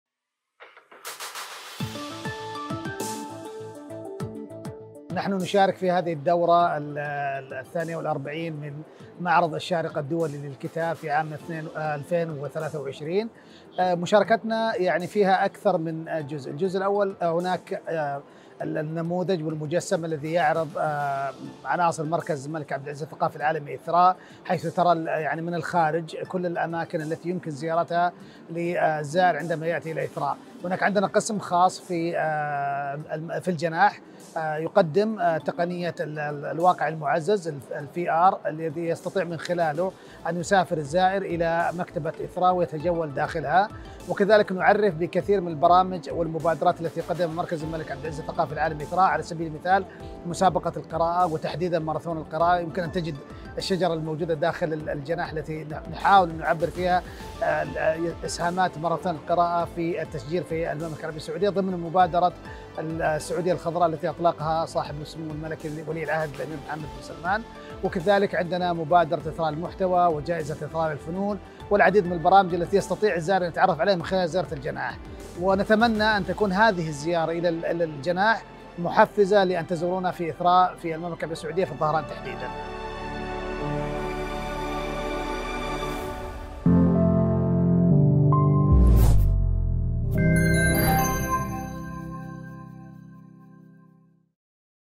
طارق الخواجة: "إثراء" يعرض مبادراته المبتكرة في الشارقة للكتاب
أوضح طارق الخواجة، المستشار الثقافي بمركز الملك عبد العزيز الثقافي العالمي "إثراء"، أن مشاركة المركز في النسخة 43 من معرض الشارقة الدولي للكتاب، تعرض نموذج مجسم لعناصر المركز.